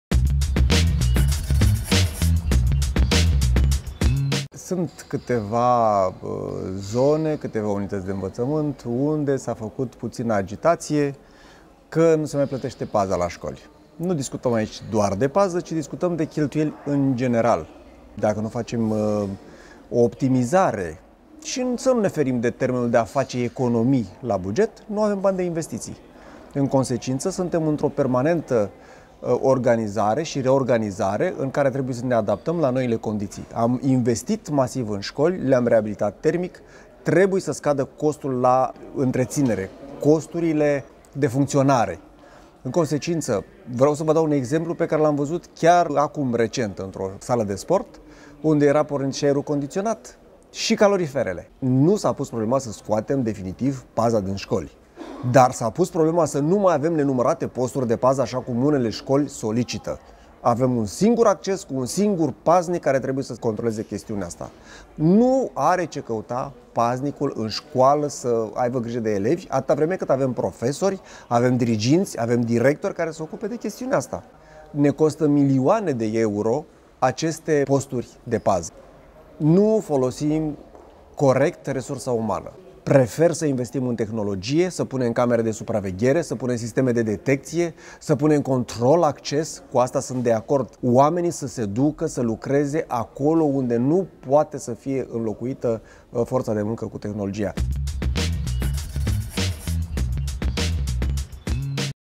I Love S3 - Sa cheltuim eficient!
Scoli modernizate si reabilitate, gradini refacute, sali de sport si terenuri reamenajate. Toate acestea s-au intamplat la unitatile de invatamant din Sectorul 3. Robert Negoita a spus inca de la inceput: Educatia este o prioritate! Aceste investitii inseamna ca, treptat, cheltuielile cu utilitatile trebuie sa scada. In paralel, evident, cu cresterea confortului pentru elevi si dascali. Sigur, este nevoie ca si comportamentul celor responsabili sa fie unul adecvat si, din pacate, nu putine sunt cazurile in care se face risipa, dand caloriferele la maximum- desi reabilitarea inseamna scaderea cheltuielilor cu pana la 40%- si lasand geamurile deschise. O alta problema este cea legata de paza. S-a zvonit, in mod eronat, ca scolile nu mai beneficiaza de acest serviciu. Fals! Sigur, insa, ca si aici, banii trebuie cheltuiti inteligent